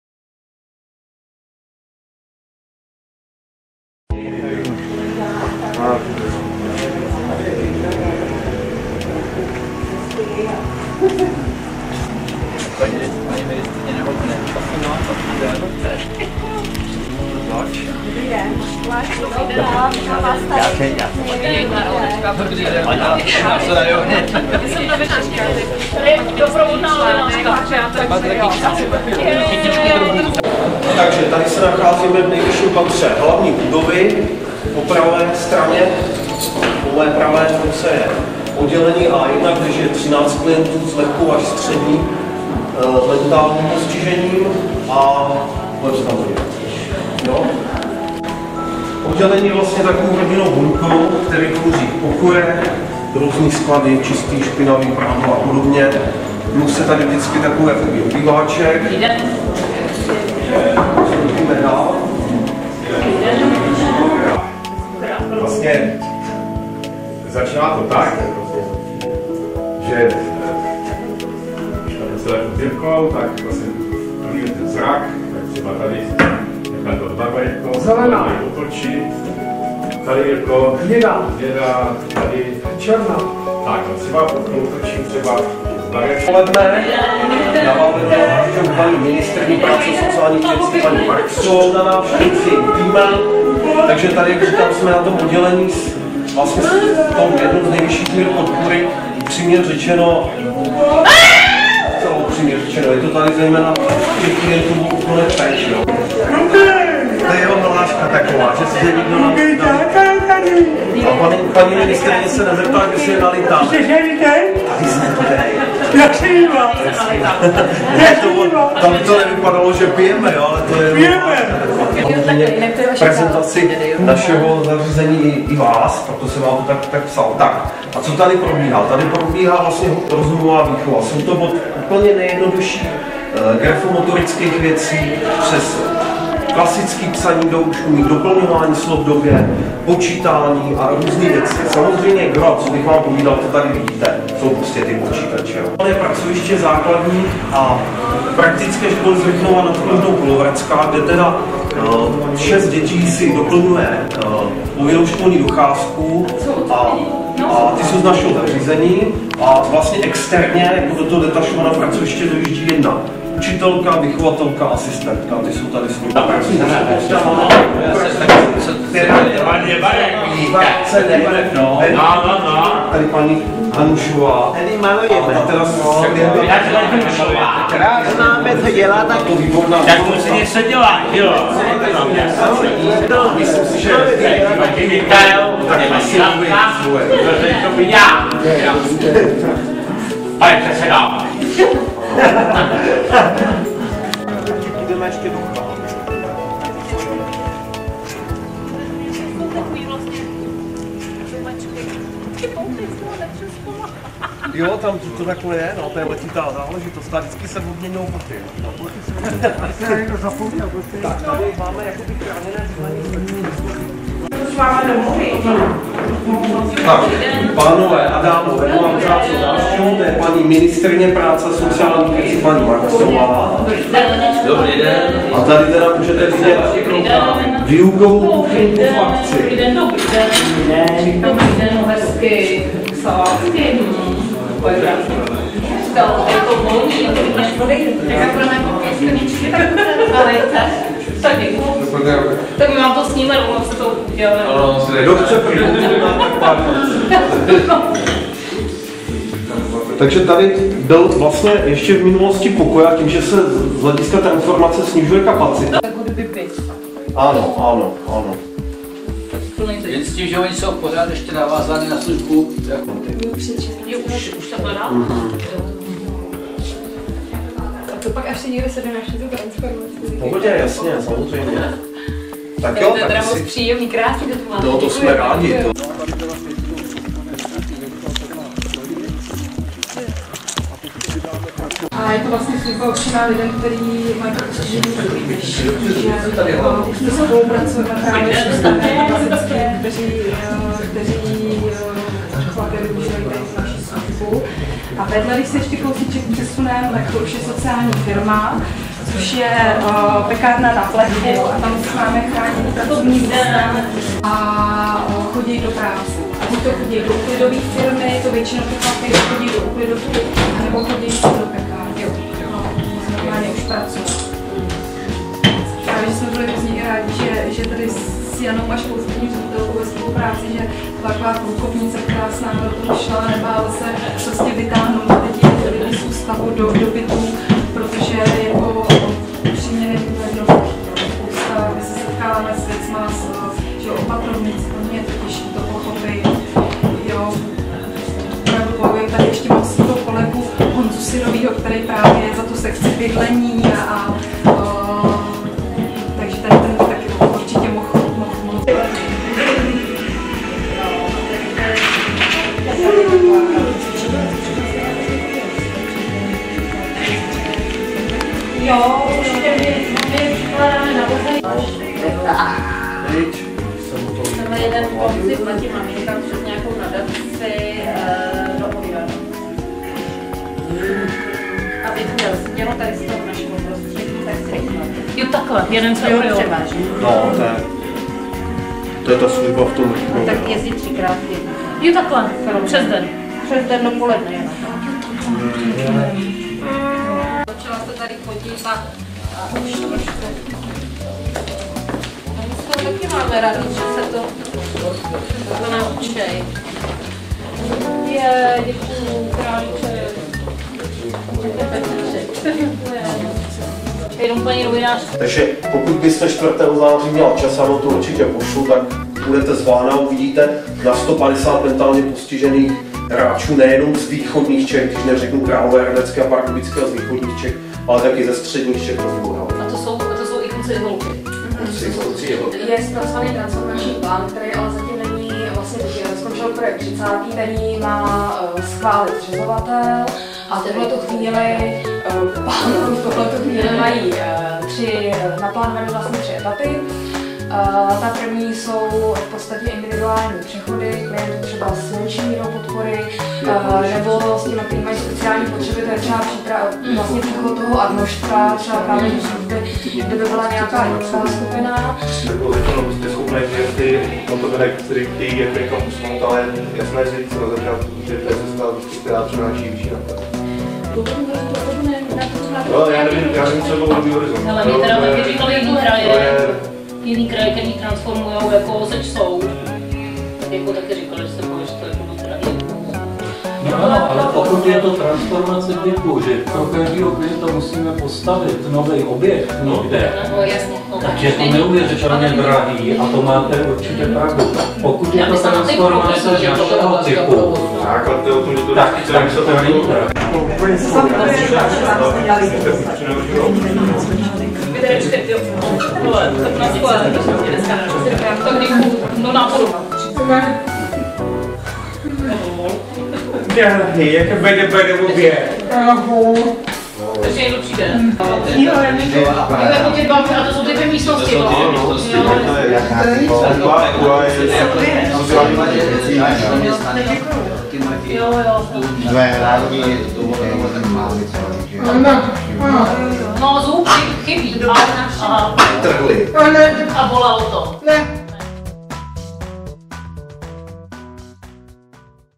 Návštěva ministryně práce a sociálních věcí ČR
Video z návštěvy ministrině práce a sociálních věcí paní Michaeli Marksové Tominové v našem zařízení.